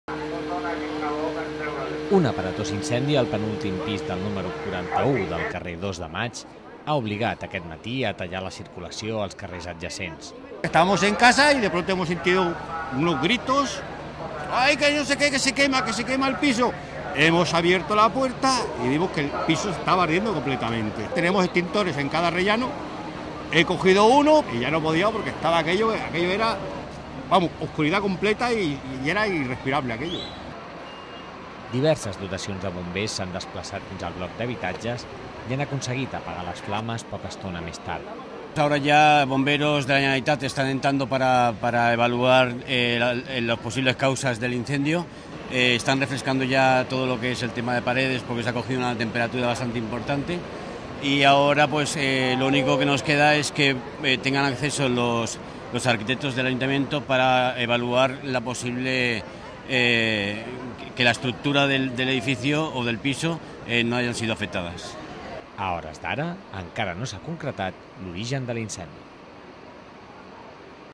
Un incendi crema un habitatge al carrer Dos de Maig
Un aparatós incendi al penúltim pis del número 41 del carrer Dos de Maig ha obligat aquest matí a tallar la circulació als carrers adjacents.

Diverses dotacions de bombers s’han desplaçat fins al bloc d’habitatges i han aconseguit apagar les flames poca estona més tard. A hores d’ara encara no s’ha concretat l’origen de l’incendi.